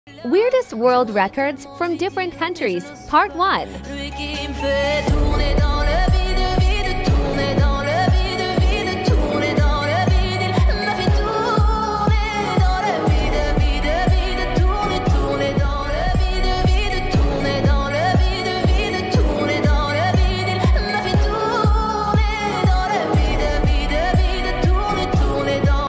Weirdest world records from different countries
Most interesting data collected and shown according to different countries using google earth animation effects.